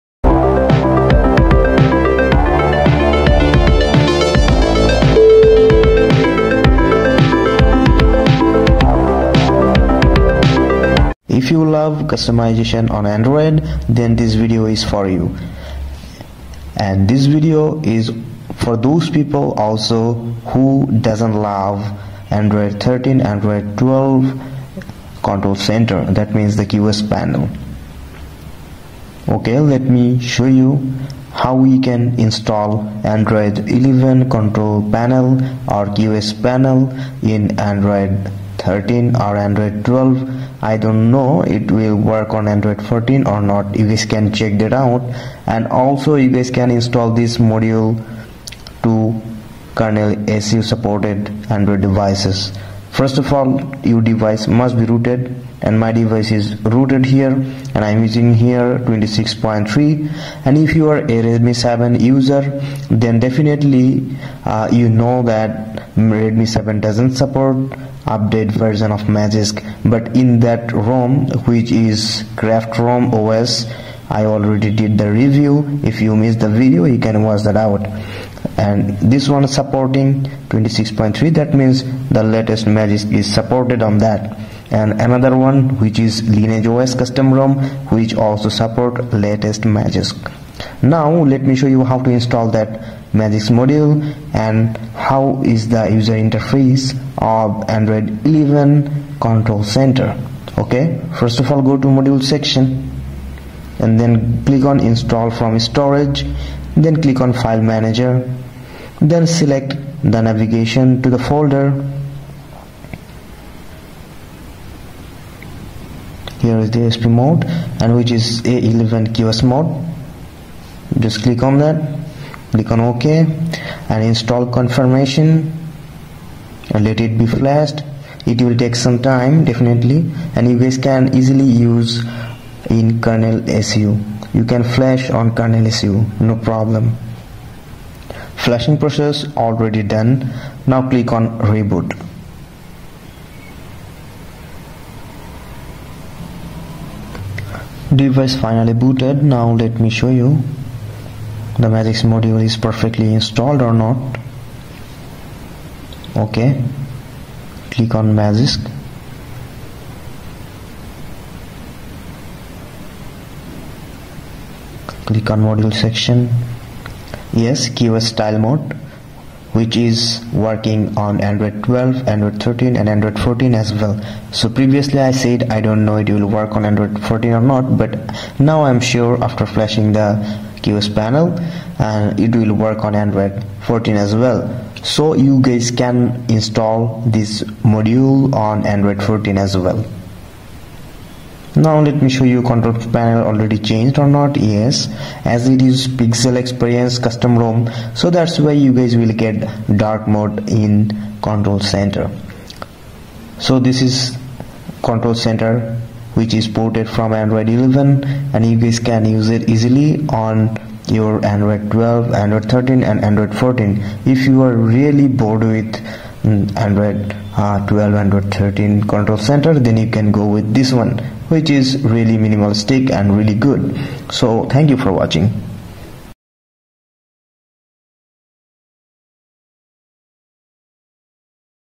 Android 11 Qs(Control Center) For Any Android 12,13 & 14|Install Android 11 Control panel All Roms
Here is the android customization mod for any android.Android 11 Qs(Control Center) For Any Android 12,13 & 14|Install Android 11 Control panel All Roms| How to install magisk module.